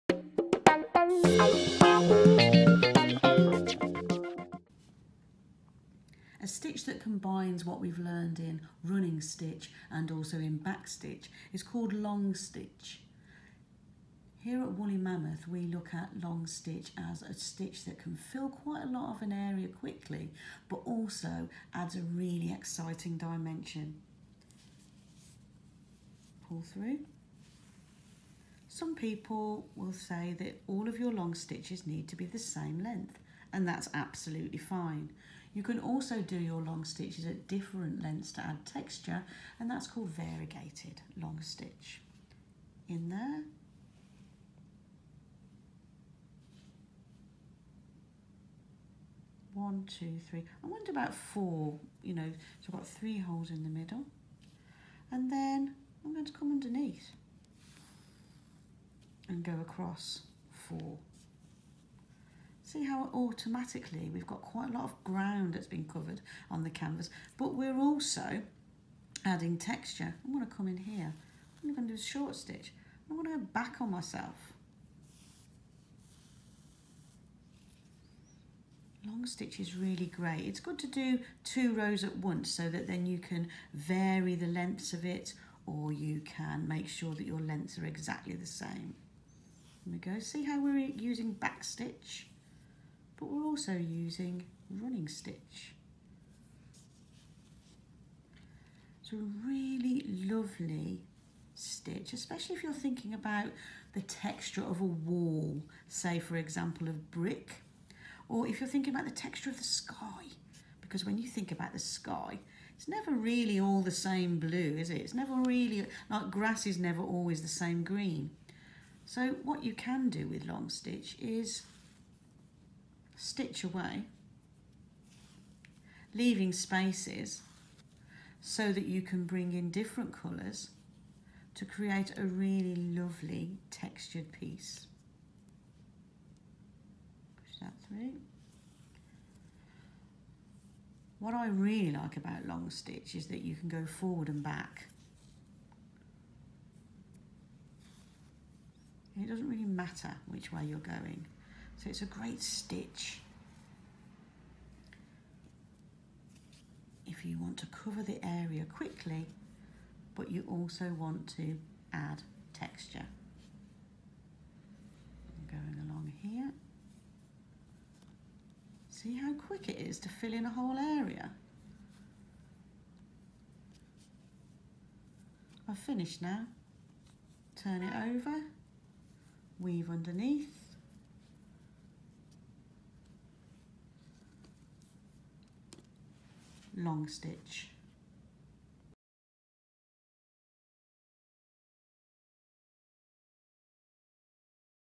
Stitch Tutorial 4: Long Stitch
Tapestry needlepoint artist Tina Francis from Woolly Mammoth Stitch Works CIC shows us how to do Long Stitch.